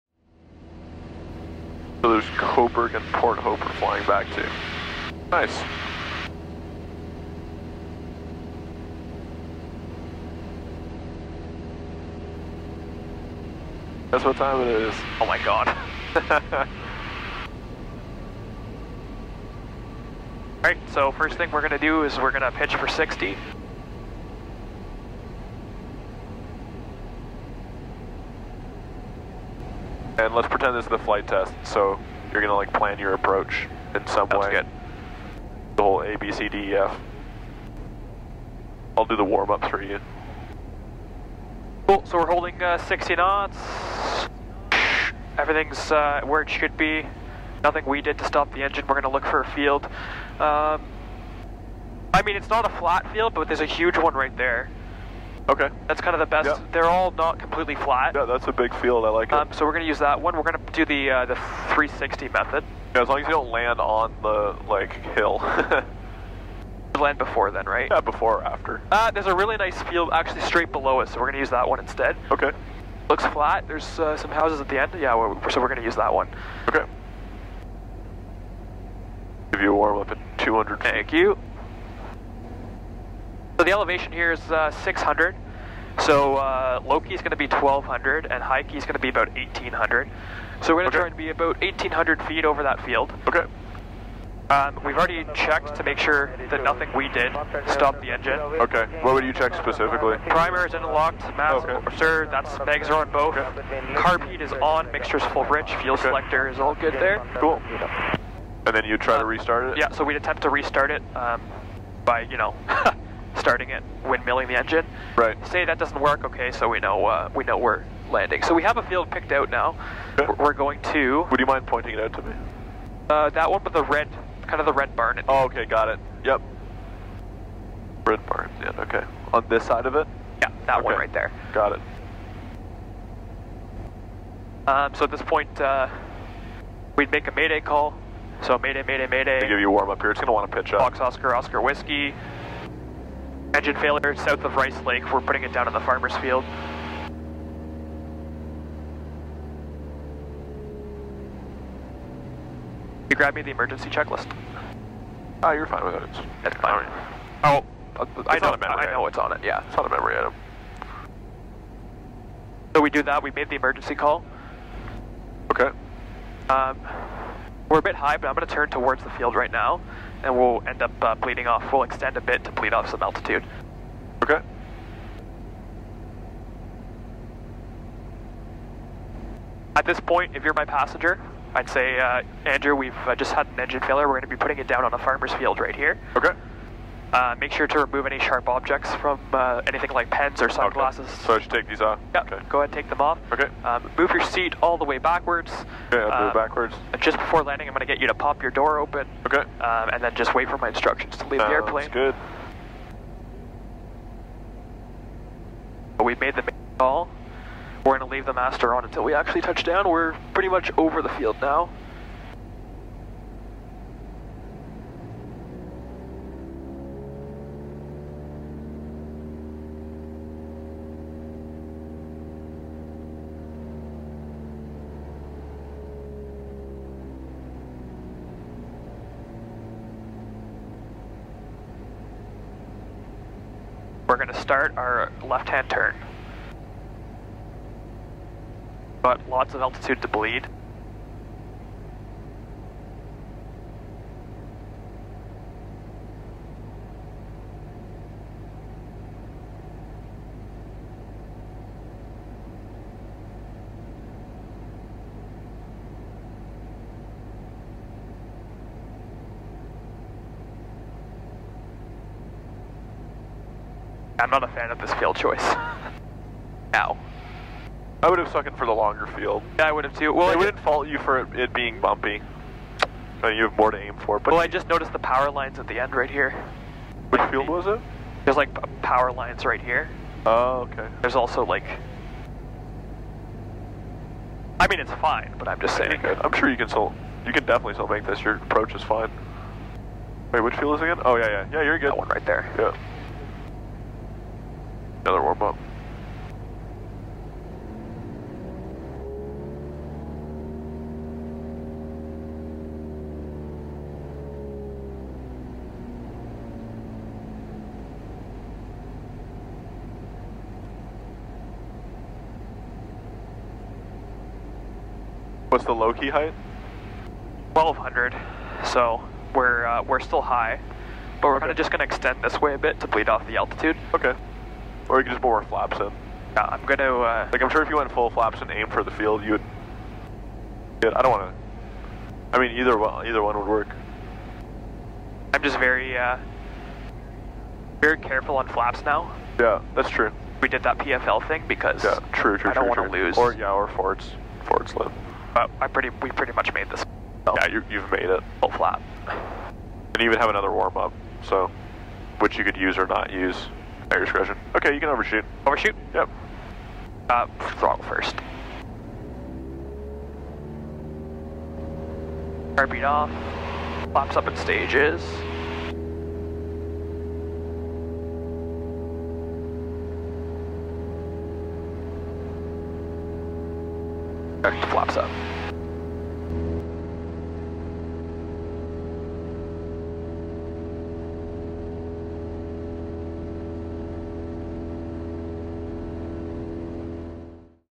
SIMULATED ENGINE FAILURE | Road to the CPL Ep.3 | Cessna 172
In this video, we practice a simulated engine failure (forced approach!) in the Cessna 172, using the 360 method. I've been practicing a ton of these recently, as my flight test is soon approaching! As soon as the power fails, we quickly identify a landing spot, secure the engine, and prepare for the approach.  Nothing better than finding the perfect field and executing a perfect approach!